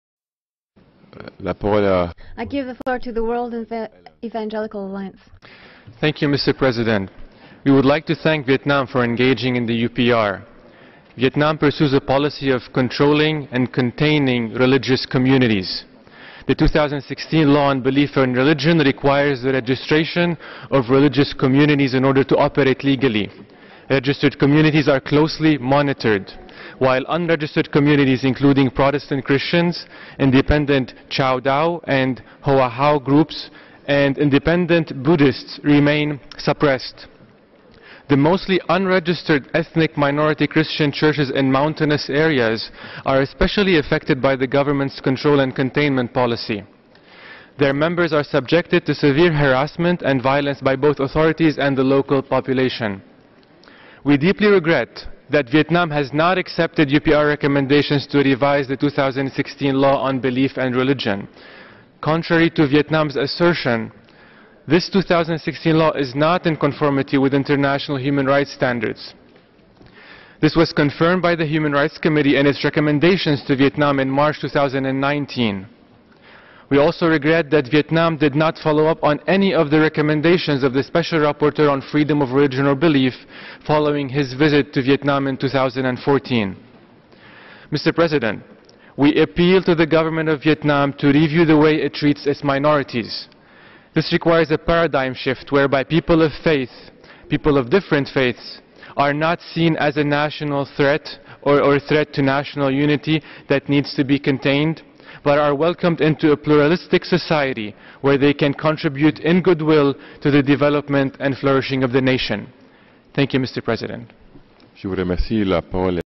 The WEA calls on Viet Nam to review the way it treats its religious minorities
The WEA appeals to the government of Viet Nam to review the way it treats its religious minorities, and to guarantee Freedom of Religion or Belief, during the adoption of the Universal Periodic Review (UPR) outcome of Viet Nam at the Human Rights Council. The statement was read by Wissam al-Saliby on 4 July 2019.